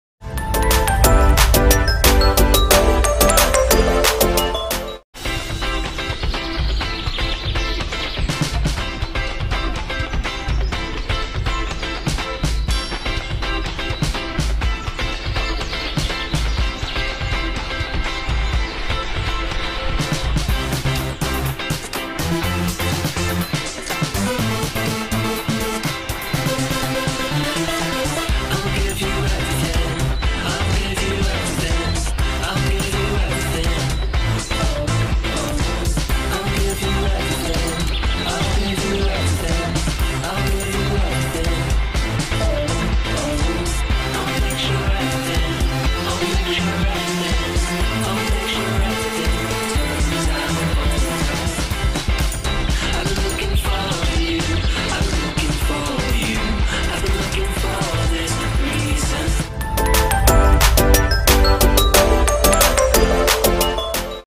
East Coast Speed Boat Trip (Customer POV) in Jersey.
we love it when our customers make videos and send them to us, flash back to our 2017 east coast speed boat trips.